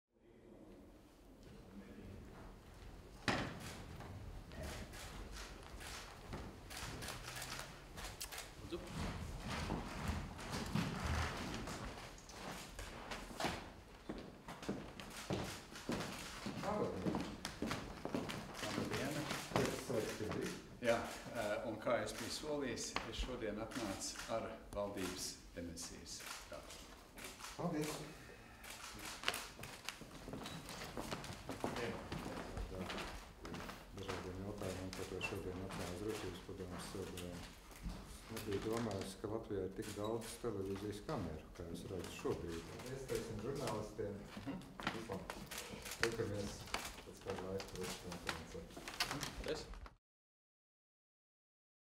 17.08.2023. Valsts prezidenta Edgara Rinkēviča tikšanās ar Ministru prezidentu Krišjāni Kariņu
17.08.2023. Valsts prezidenta Edgara Rinkēviča tikšanās ar Ministru prezidentu Krišjāni Kariņu. Video autors: Gatis Bergmanis, Valsts prezidenta kanceleja.